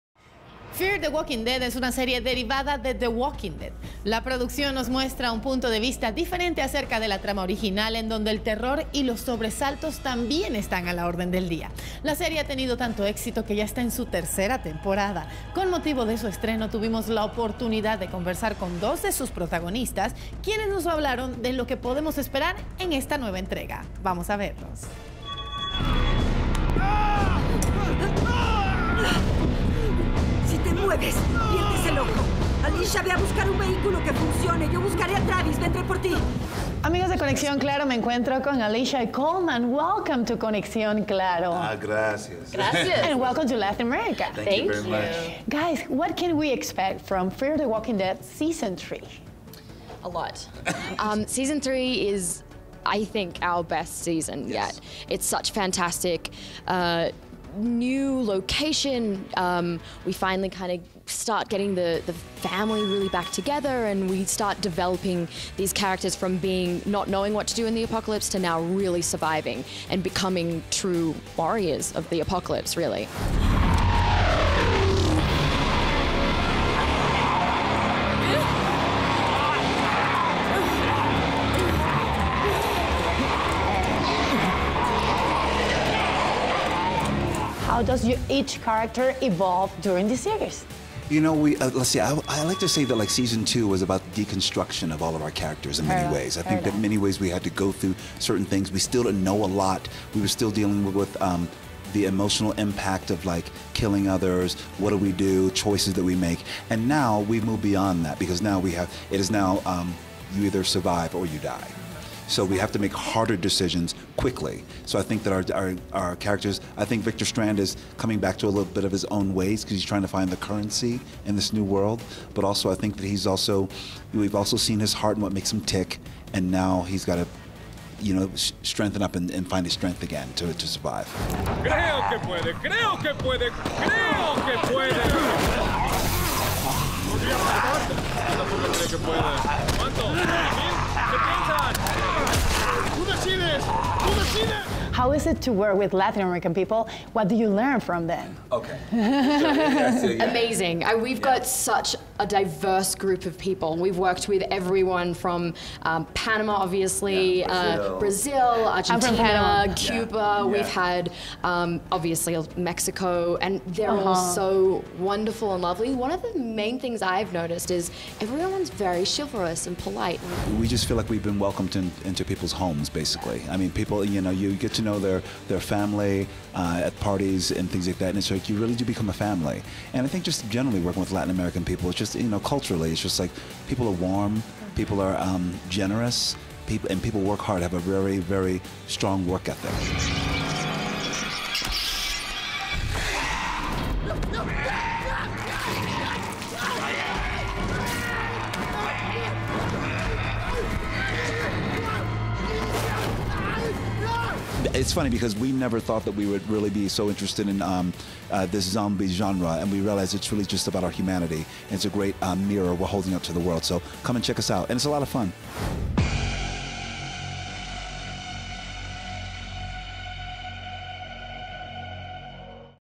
CONEXIÓN Claro - Fear The Walking Dead S3: Alycia Debnam-Carey & Colman Domingo Interview
Alycia Debnam-Carey as Alicia Clark